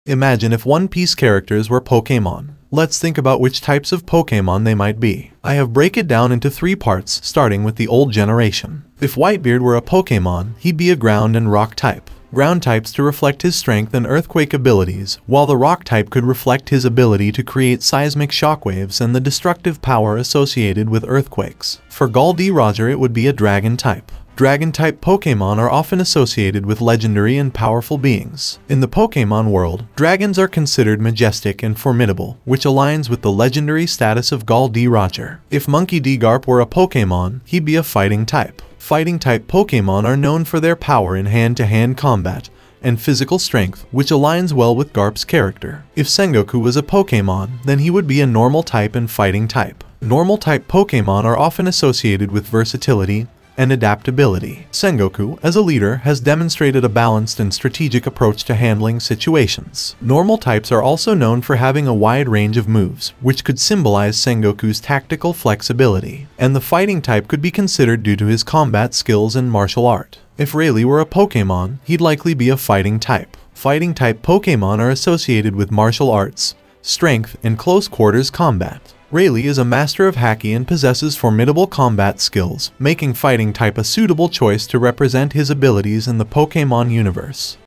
ONE PIECE | WHAT IF ONE PIECE CHARACTER WERE A POKEMON!
If One Piece characters were Pokémon, what type of Pokémon would they be?

@weebmafia   Follow this channel for amazing One Piece Posts.